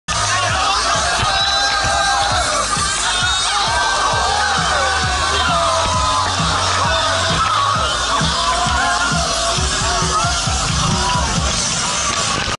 Breakdancer kicks a girl at Arenal sound 2016
El chico está efectuando varios backflips a través de un pasillo humano cuando una chica entra sin mirar.